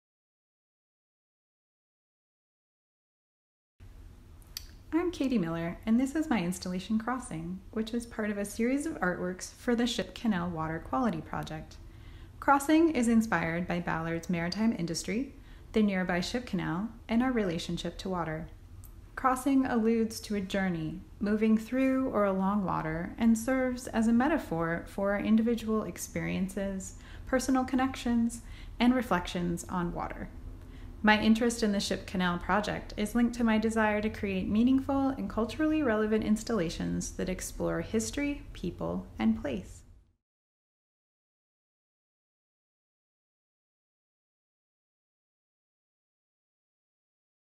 "Crossing" by Katie Miller
Aligned with the goals outlined in the Ship Canal Water Quality Project Drainage and Wastewater Master Plan by Vaughn Bell, the artworks seek to make the invisible visible and to provide visitors with an understanding of the flow of water on site and through the surrounding neighborhoods.

Crossing is inspired by Seattle’s maritime industry, the nearby ship canal, and our relationship to water. Crossing alludes to a journey moving through or along water and serves as a metaphor for our individual experiences, personal connections to, and reflections on water.